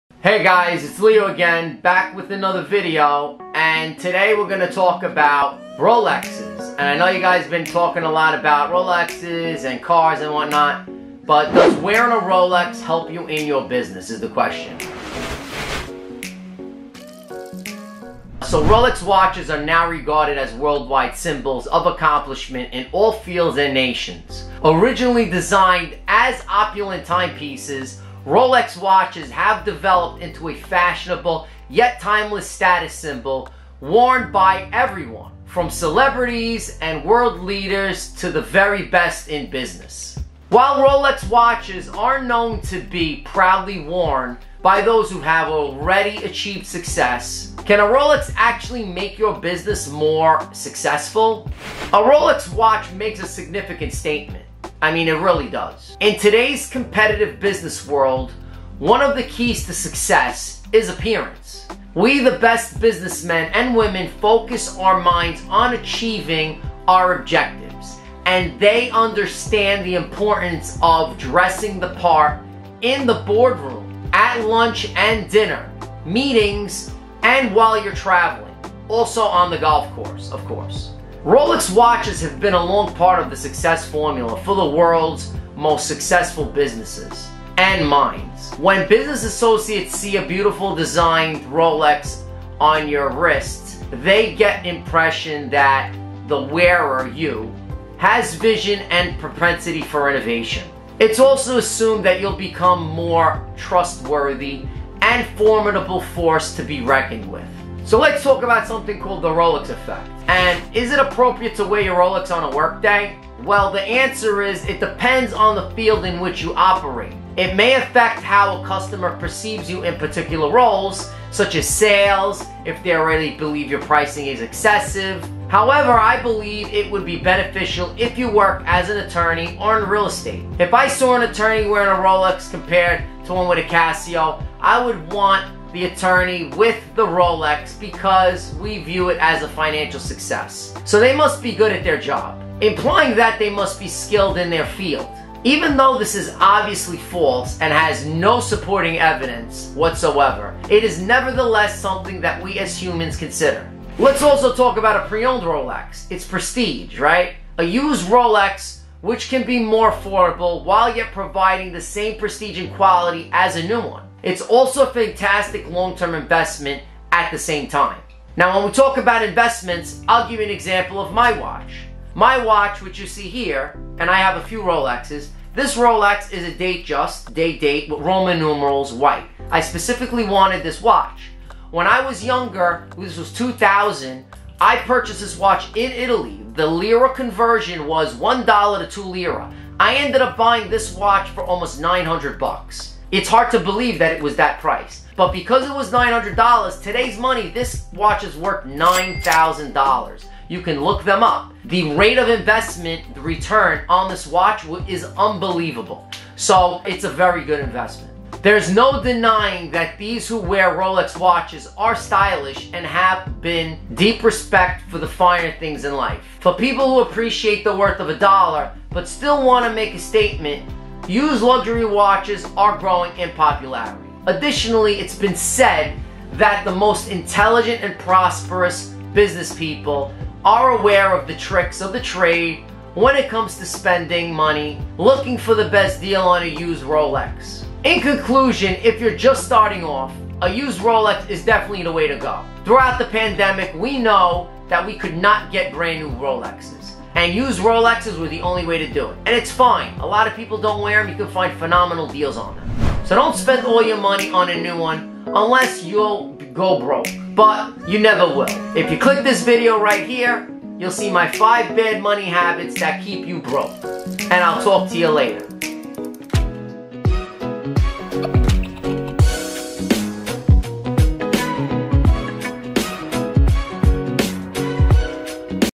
Does Wearing A Rolex Help You In Your Business?
Why Are Rolex Watches So Expensive?

Rolex watches are now regarded as worldwide symbols of accomplishment in all fields and nations.

In this video, we cover: Is Rolex a good investment, Are Rolex watches a good investment, and Are Rolex watches worth the money?